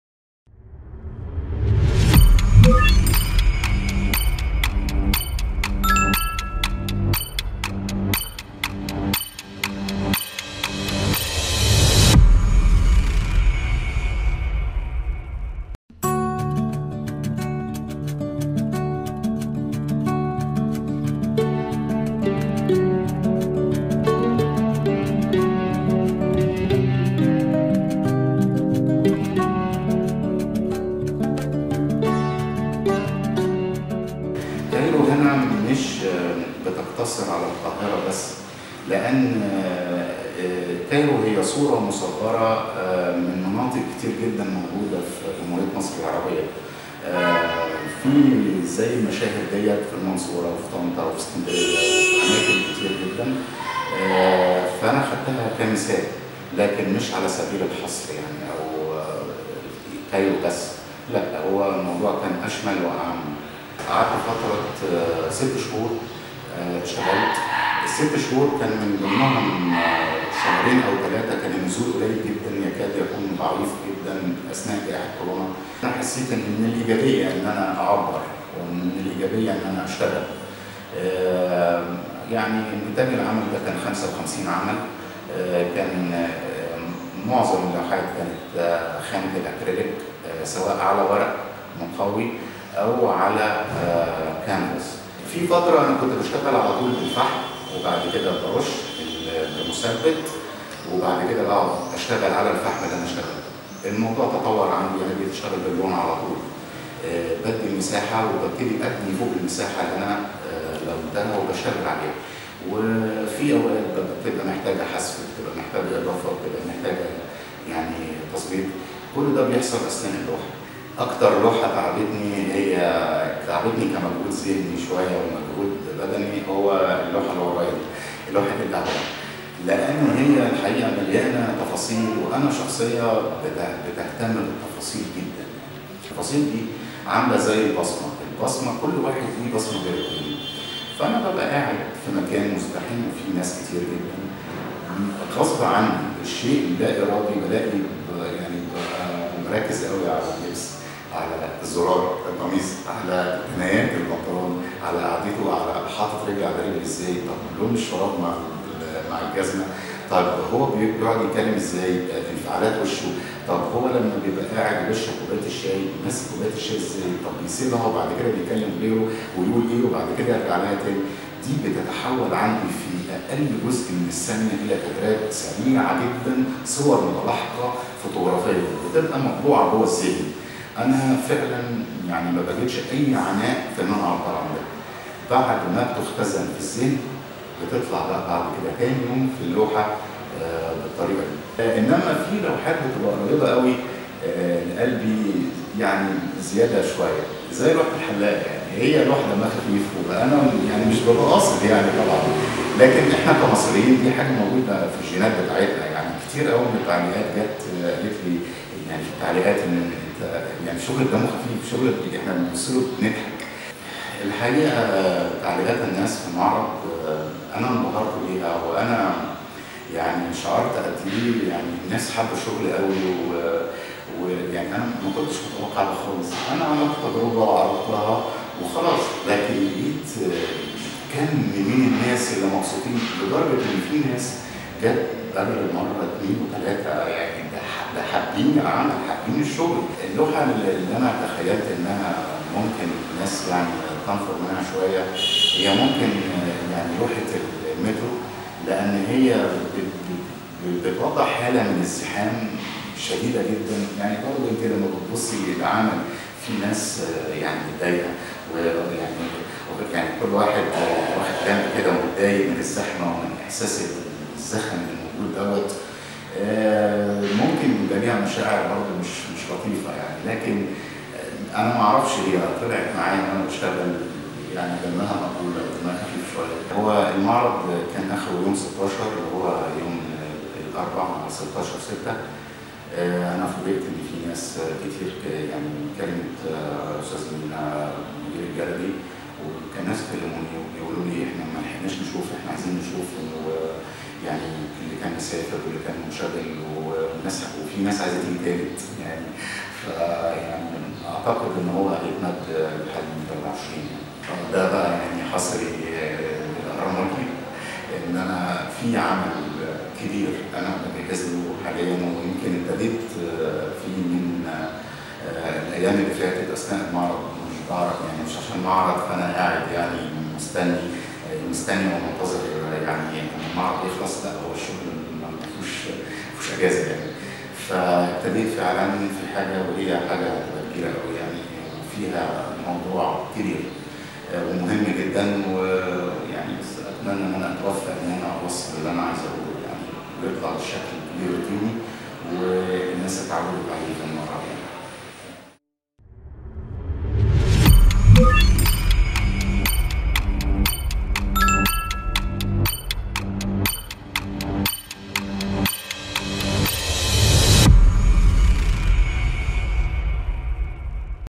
Cairo exhibition @ Access Art Gallery
Access art space presents "Cairo" a solo exhibition by the artist Joseph El-Duwairy which includes 55 paintings.
The exhibition started 22nd May 2021 and will extended until 23rd June.
Visiting hours: from 12 pm to 9 pm except Thursdays and Fridays.
adress: 10 El Nabarawy St- Mohamoud Bassiouny St. Downtown - Cairo.

Egypt’s imponderable industry
Rania Khallaf explores new visions of Cairo